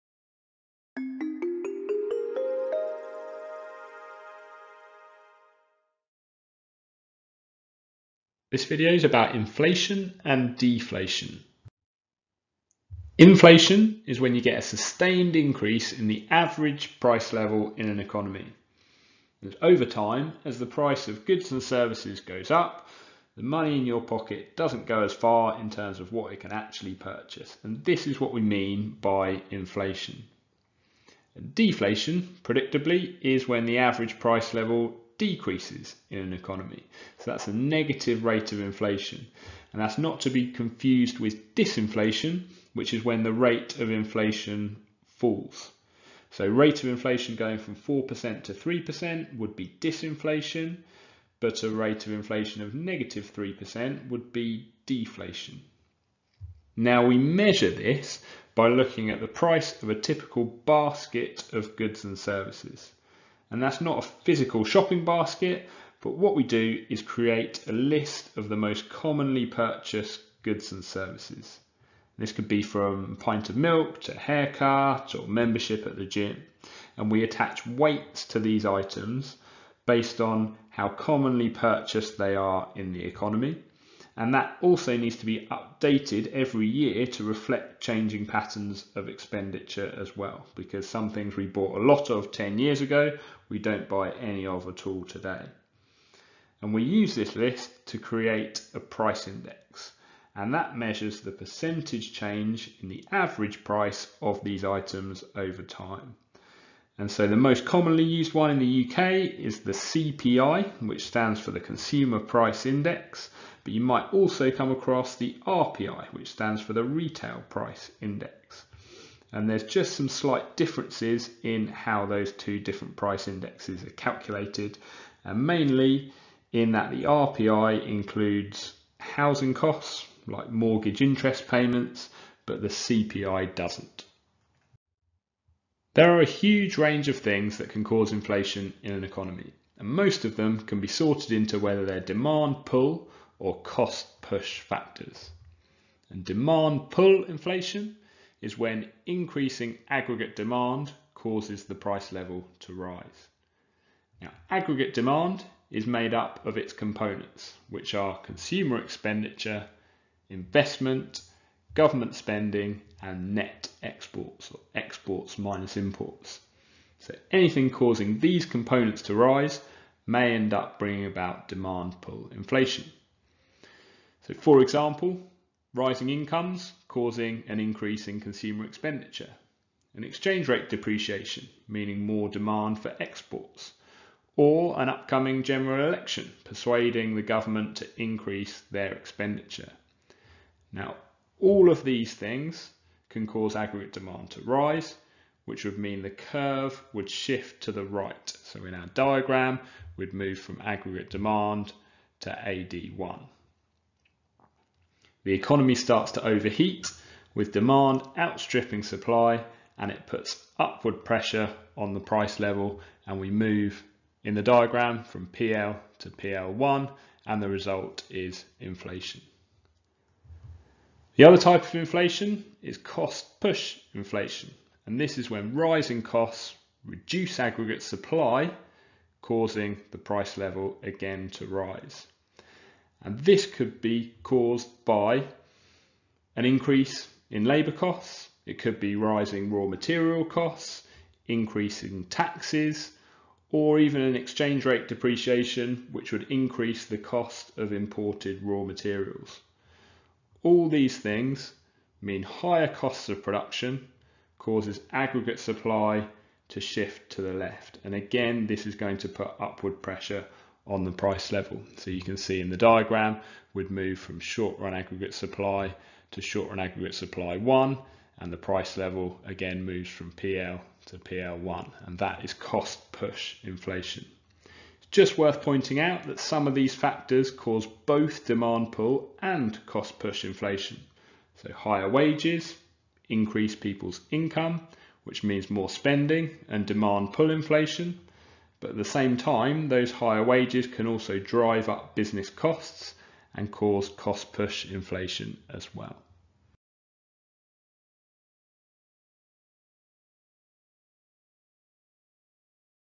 Inflation and Deflation
Inflation is a sustained increase in the price level in an economy. It is important to be able to distinguish between deflation (negative rates of inflation) and disinflation (falling rates of inflation). Causes of inflation can be sorted into demand pull and cost push factors